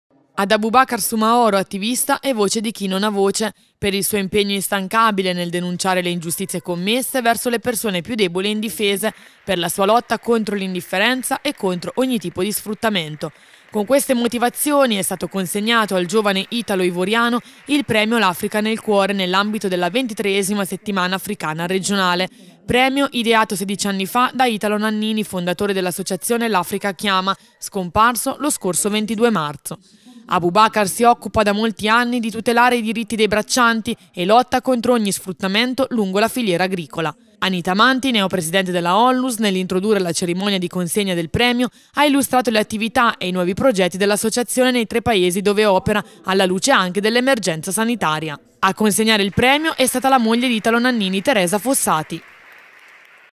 Consegnato il premio "Ho l'Africa nel Cuore"
Giovedì 1 ottobre, in occasione della XXIII Settimana Africana Regionale, è stato consegnato il premio "Ho l'Africa nel Cuore" a a Aboubakar Soumahoro, attivista italo-ivoriano impegnato sindacalmente a favore delle persone deboli,  indifese e spesso invisibili